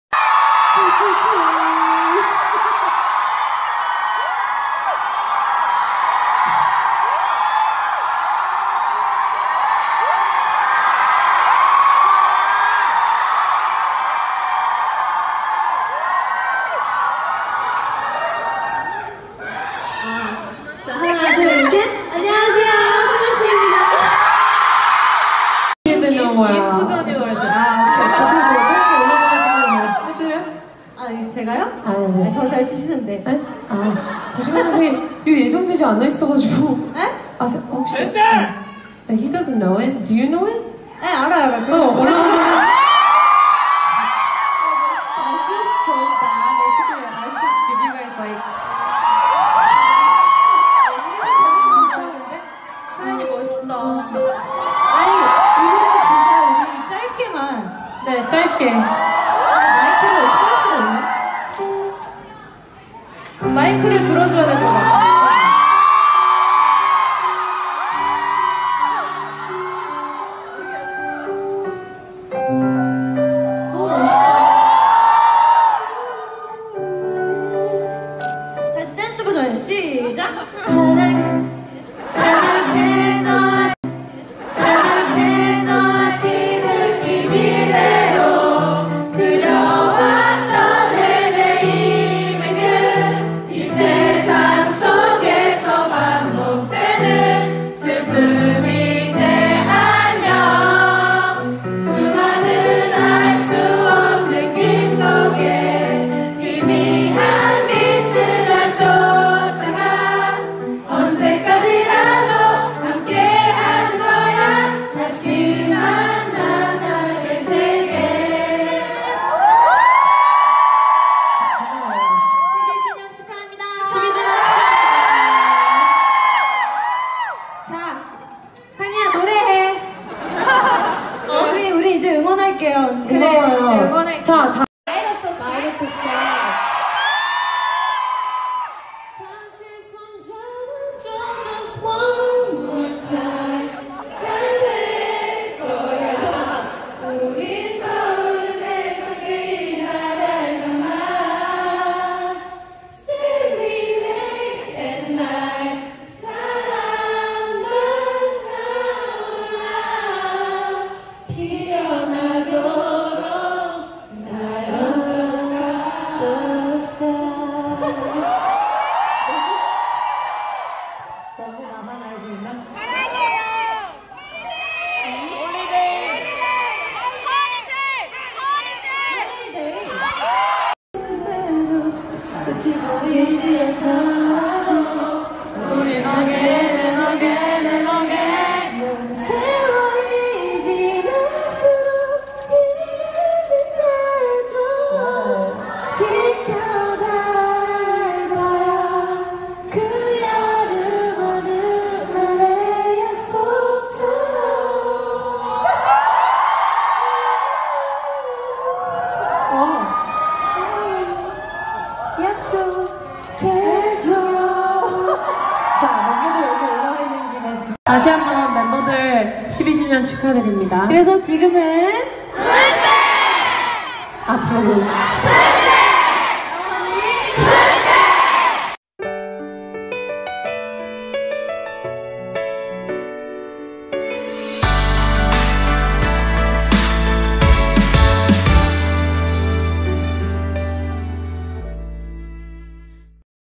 Tiffany - Open Hearts Eve Concert with Sooyoung & Seohyun
Open Hearts Eve Concert
Tiffany Young, Sooyoung,  Seohyun SNSD
2019 August  03